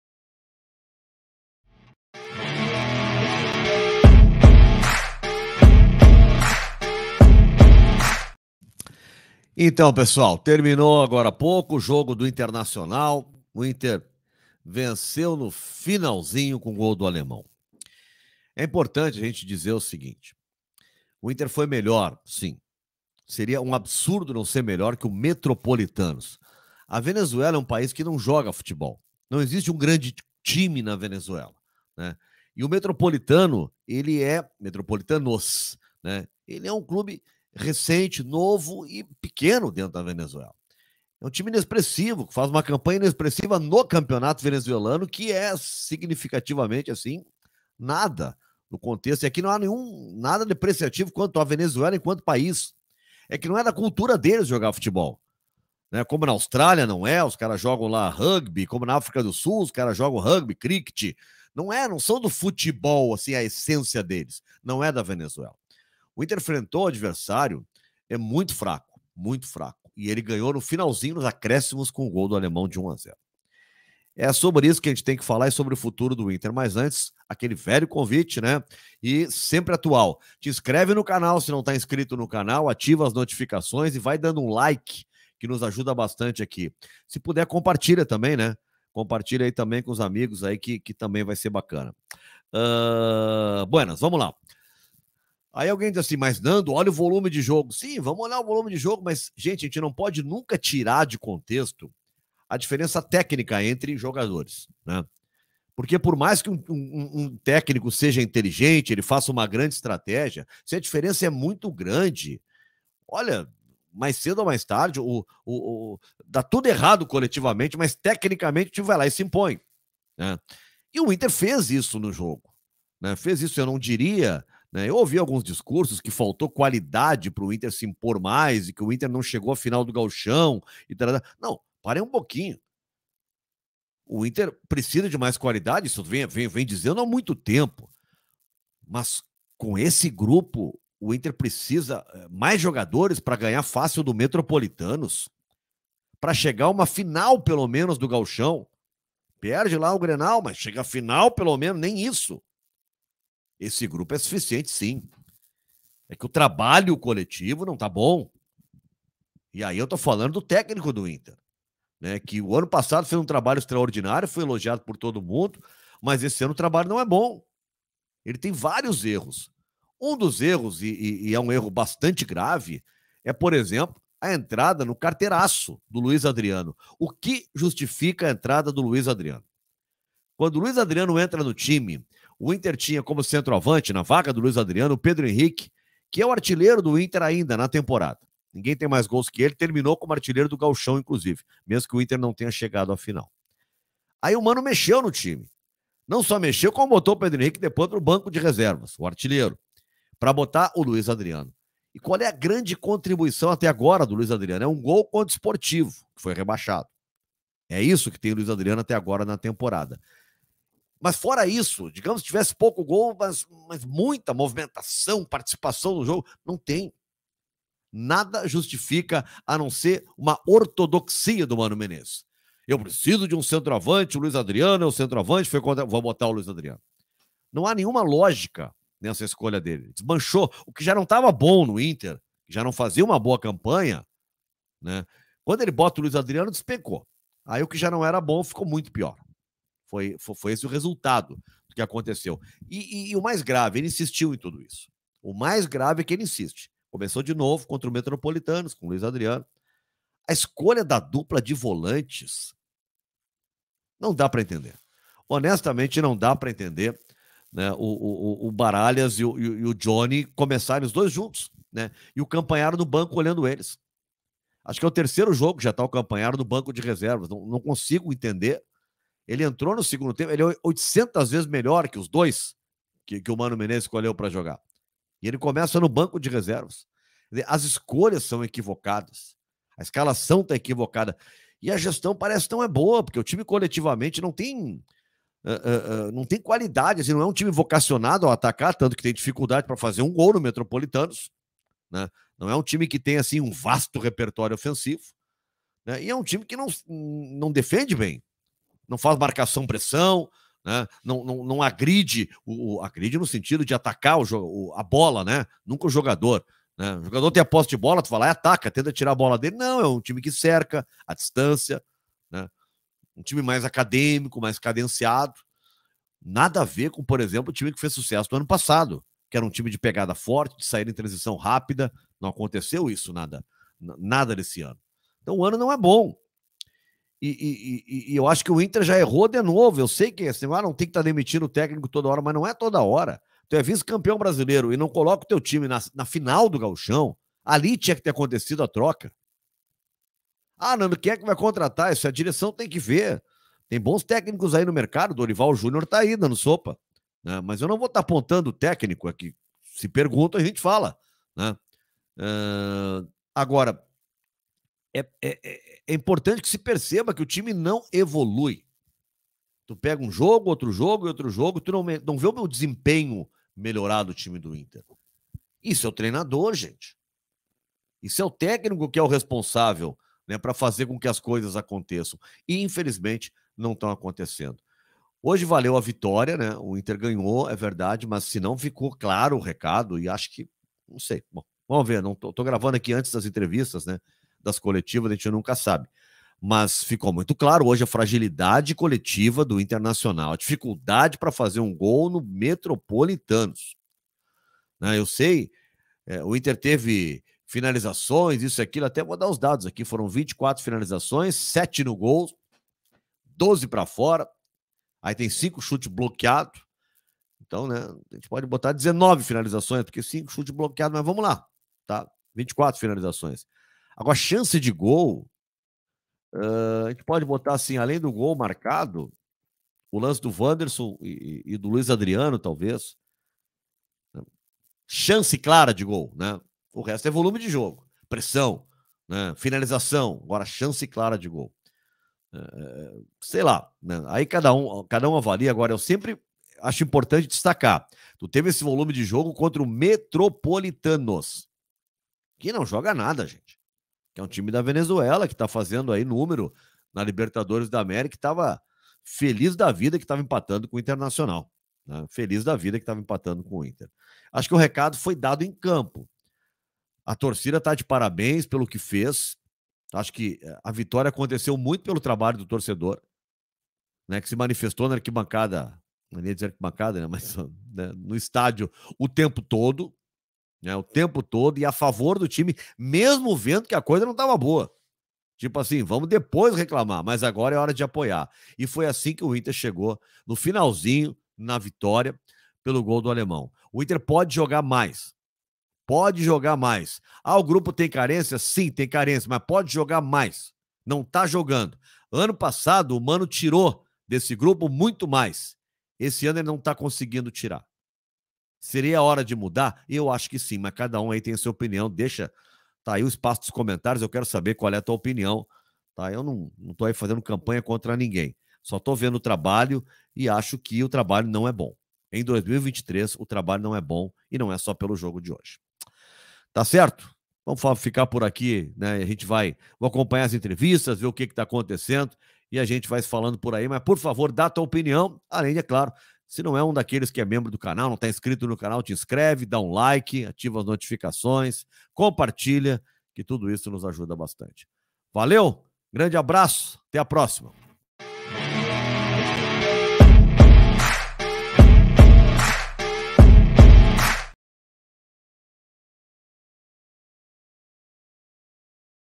INTER VENCE NO SUFOCO O METROPOLITANOS - DESEMPENHO É PREOCUPANTE
Inter chegou à vitória com gol nos acréscimos de Alemão.

Arte - @rafaelbrum01

Renus - Soluções em Metais e Plásticos Telefone: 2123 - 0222